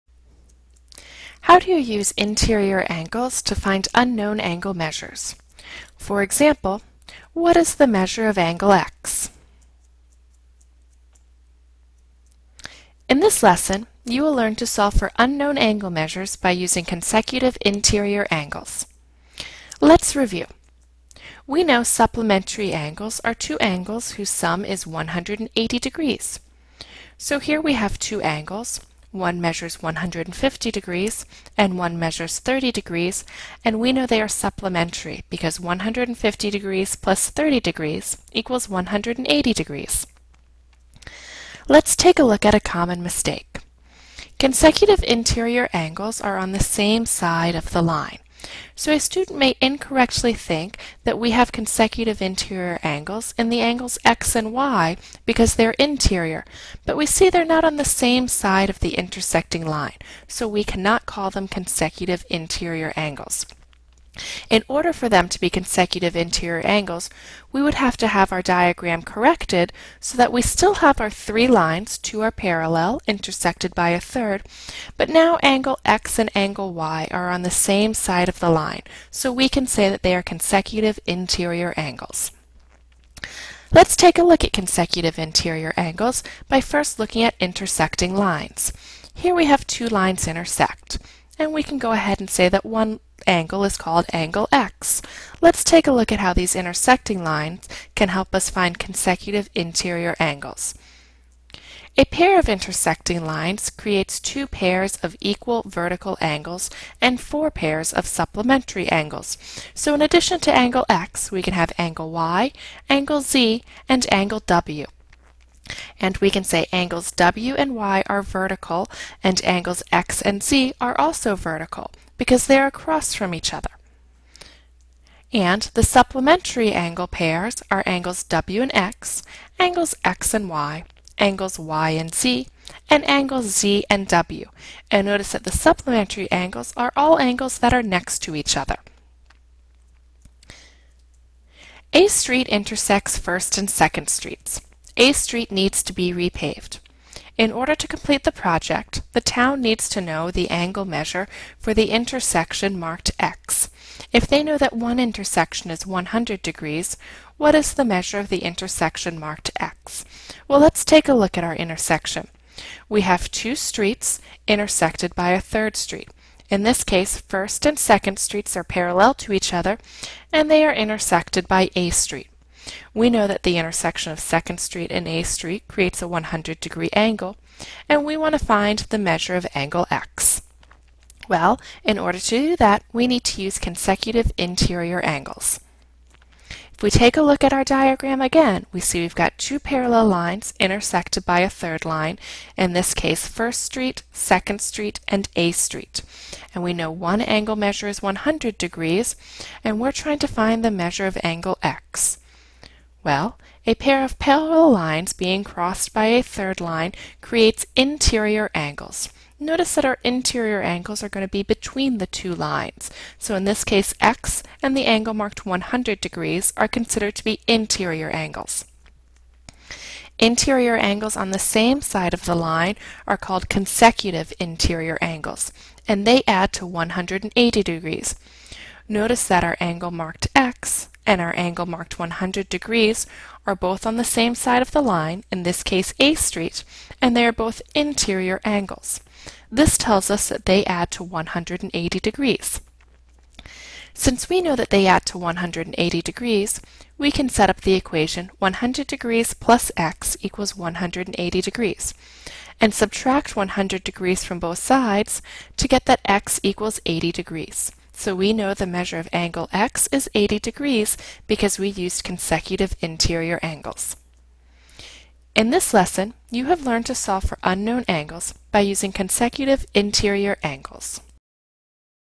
Use consecutive interior angles to find unknown angles
In this lesson you will learn to solve for unknown angle measures by using consecutive interior angles.

ADDITIONAL MATERIALS

STANDARDS

CCSS.7.G.B.5 Use facts about supplementary, complementary, vertical, and adjacent angles in a multi-step problem to write and solve simple equations for an unknown angle in a figure.

TEKS.7.11.C write and solve equations using geometry concepts, including the sum of the angles in a triangle, and angle relationships.

TEKS.G.5.A investigate patterns to make conjectures about geometric relationships, including angles formed by parallel lines cut by a transversal, criteria required for triangle congruence, special segments of triangles, diagonals of quadrilaterals, interior and exterior angles of polygons, and special segments and angles of circles choosing from a variety of tools;

IN.7.GM.4 Solve real-world and other mathematical problems that involve vertical, adjacent, complementary, and supplementary angles.

VA.MG.8.5 The student will use the relationships among pairs of angles that are vertical angles, adjacent angles, supplementary angles, and complementary angles to determine the measure of unknown angles.

FL.MAFS.7.G.2.5 Use facts about supplementary, complementary, vertical, and adjacent angles in a multi-step problem to write and solve simple equations for an unknown angle in a figure.